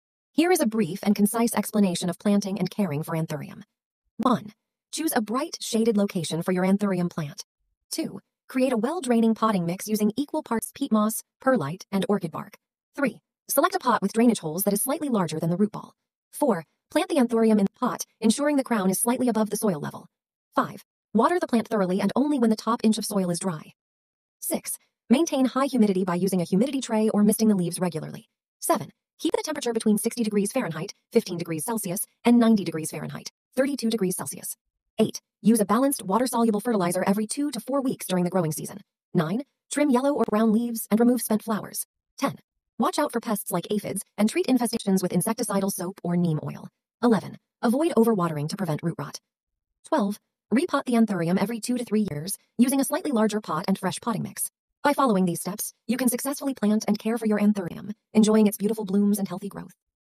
Steps to plant and care for Anthurium Andraeanum.
Planting and caring for anthurium involves several steps to ensure the health and beauty of the plant. Here's a guide to help you:

1. Choose a suitable location: Anthuriums thrive in bright, indirect light. Select a location with filtered or shaded sunlight, away from direct exposure to harsh sunlight.

2. Prepare the potting mix: Anthuriums prefer well-draining soil. Create a potting mix by combining equal parts of peat moss, perlite, and orchid bark. This mixture provides adequate aeration and moisture retention.

3. Select an appropriate container: Choose a pot with drainage holes to prevent waterlogging. Anthuriums prefer tight spaces, so select a pot that is only slightly larger than the root ball.

5.Planting the anthurium:
a. Fill the bottom of the pot with a layer of potting mix.
b. Gently remove the anthurium from its nursery pot, taking care not to damage the roots.
c. Place the anthurium in the pot and fill the sides with the potting mix, ensuring that the crown (where the leaves emerge from the soil) is slightly above the soil level.
d. Lightly press the soil around the plant to secure it.

6. Watering:
a. Water the anthurium thoroughly after planting until excess water drains out of the pot.
b. Water the plant when the top inch of soil feels dry to the touch. Anthuriums prefer slightly moist soil but can't tolerate standing water.
c. Avoid overwatering, as it can lead to root rot.

7. Humidity and temperature:
a. Anthuriums prefer high humidity. Place a humidity tray filled with water near the plant or mist the leaves regularly.
b. Maintain a temperature between 60°F (15°C) and 90°F (32°C) for optimal growth.

8. Fertilizing:
a. Use a balanced, water-soluble fertilizer formulated for flowering plants.
b. Dilute the fertilizer to half the recommended strength and apply it every two to four weeks during the growing season (spring and summer).
c. Reduce fertilization frequency during the dormant period (fall and winter).

9. Pruning:
a. Trim yellow or brown leaves near the base to maintain plant health and appearance.
b. Remove spent flowers by cutting the stem back to the base to encourage new growth.

10. Pests and diseases:
a. Keep an eye out for common pests like aphids, mealybugs, and spider mites. Treat infestations with insecticidal soap or neem oil.
b. Anthuriums are generally disease-resistant but can be susceptible to root rot if overwatered. Ensure proper drainage and avoid waterlogging.

11. Repotting:
a. Repot the anthurium every two to three years or when the plant outgrows its current pot.
b. Choose a pot one size larger and use fresh potting mix.
c. Carefully remove the plant from its current pot, trim any damaged roots, and replant it in the new pot.

Remember to observe your anthurium regularly and adjust the care as needed. With proper attention, your anthurium will flourish and reward you with its stunning blooms.